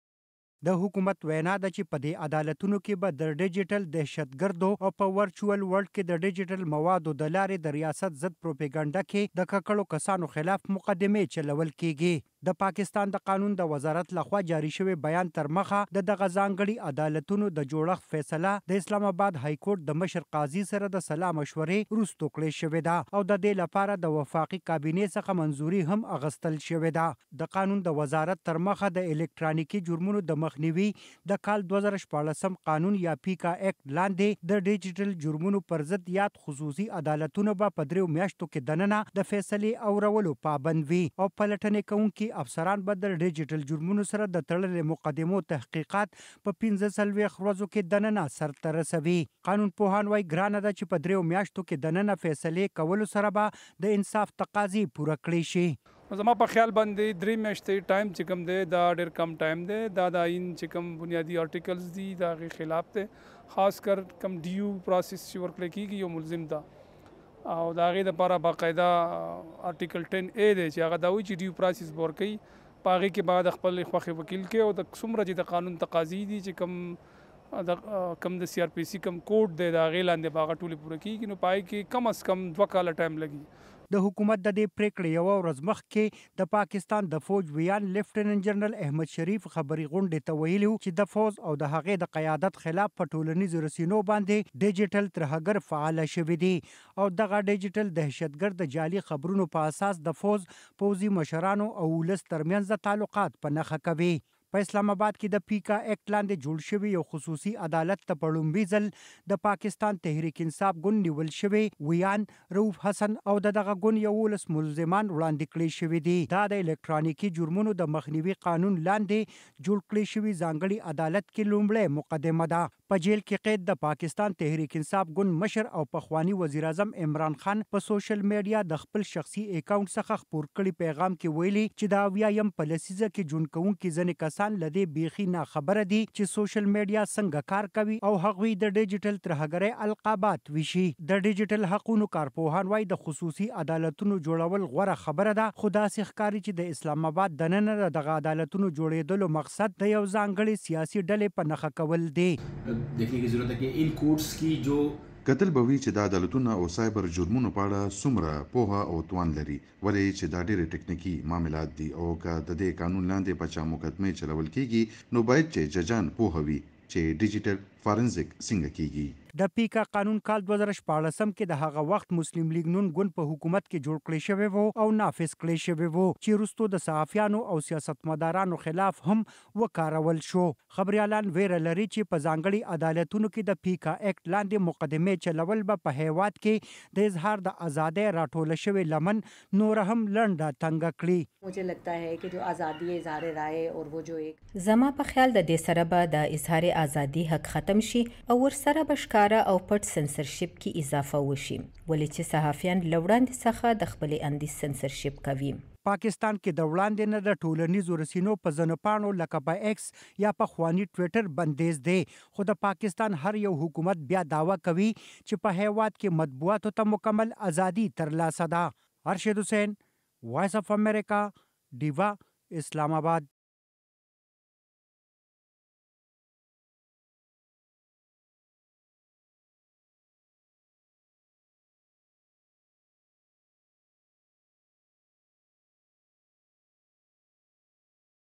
په انټرنېټ د جرمونو خلاف د پاکستان وړمبي عدالت کار شروع کړو
د پاکستان وفاقي حکومت په ټولنيزو رسنيو باندې د رياستي ادارو او د هغې د مشرانو خلاف د پروپېګنډې او کرکه خوروونکو عناصرو د محاکمه کولو لپاره د پيکا اېکټ لاندې خصوصي عدالتونه جوړ کړي او دغه عدالتونو د نن څخه خپل کار پېل کړی دی. تفصيل د ارشد حسېن په رپورټ کې: